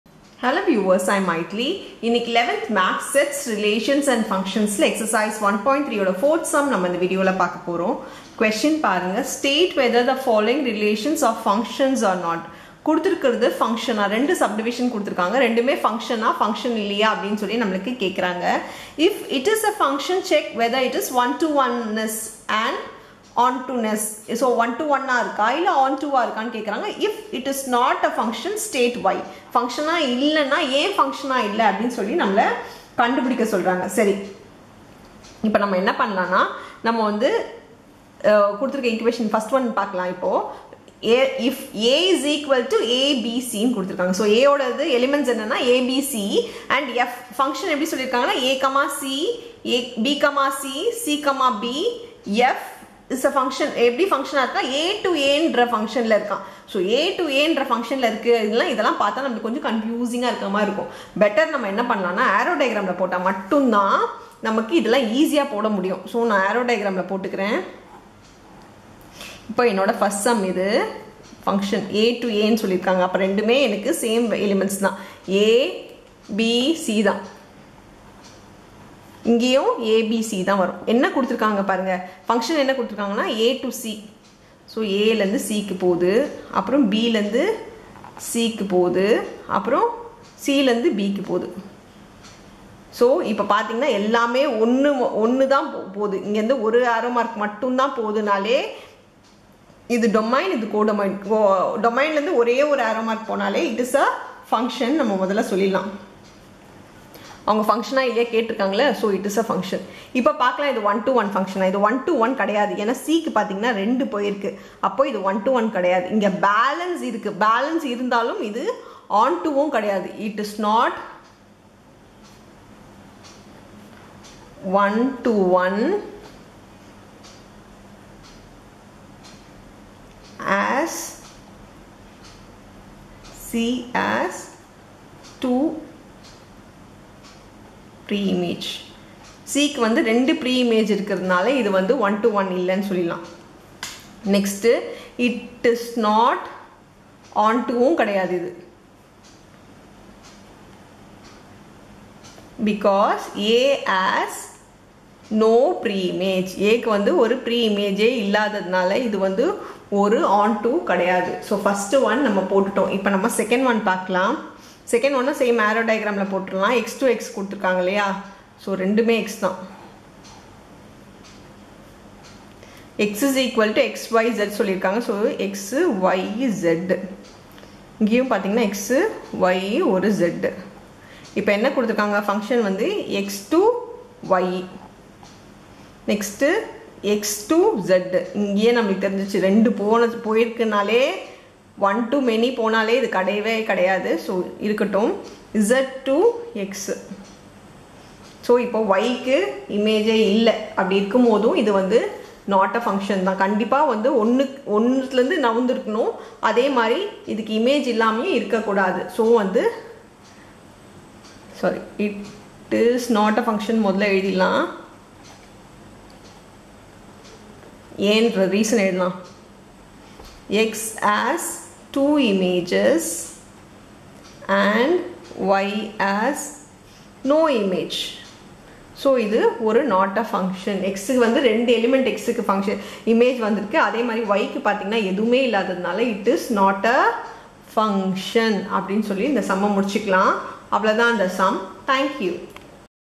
TN 11th Maths Sets, Relations & Functions Ex 1.3 Sum 4
Tamil Nadu State Board 11th Maths Complete Solutions For Ex 1.3 Sum 4 In Sets, Relations & Functions.